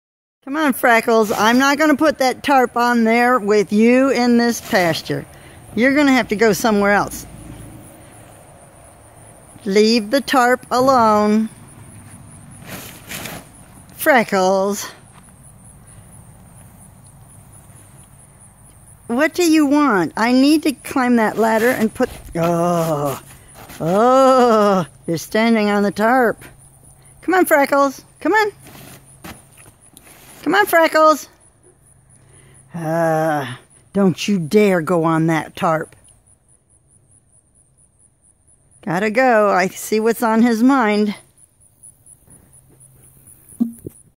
Freckles at it again!
Freckles messing with the tarp